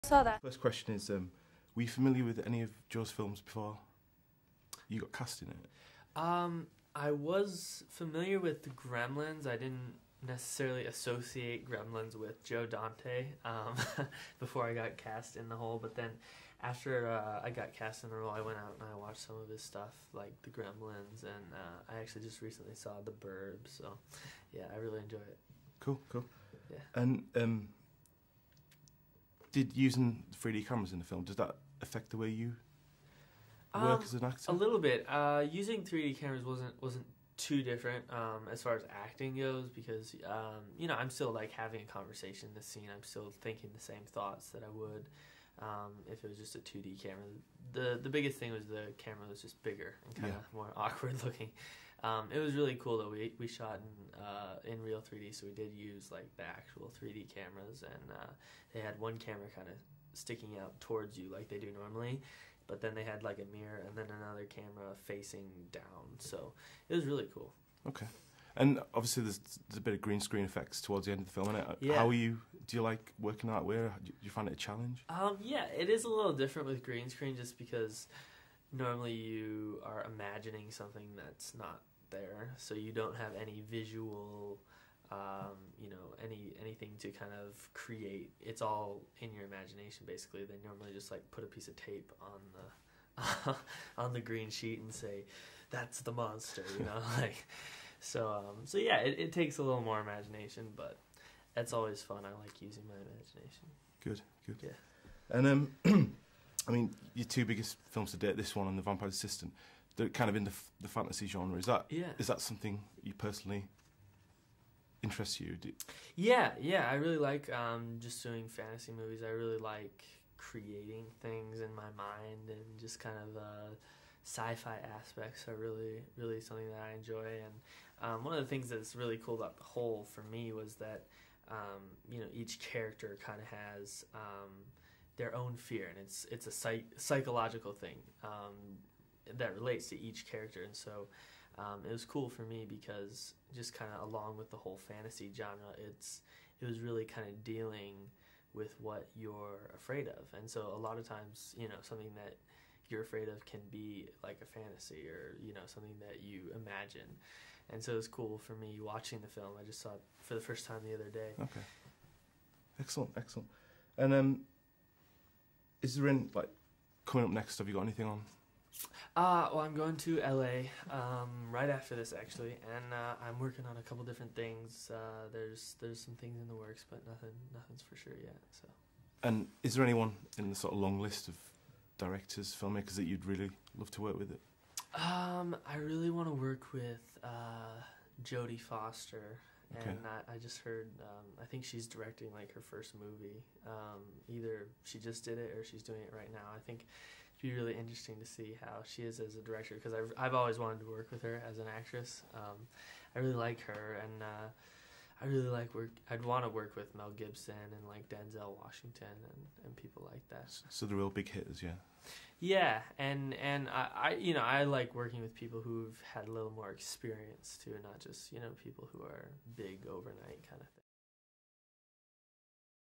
Chris Massoglia - The Hole In 3D Exclusive Interview
Adam Lowes from HeyUGuys sits down with star of Joe Dante's The Hole In 3D, Chris Massoglia to talk about making the movie and working with the legendary director.

Plot: Two brothers find a hole in the basement that leads to their darkest fears and nightmares.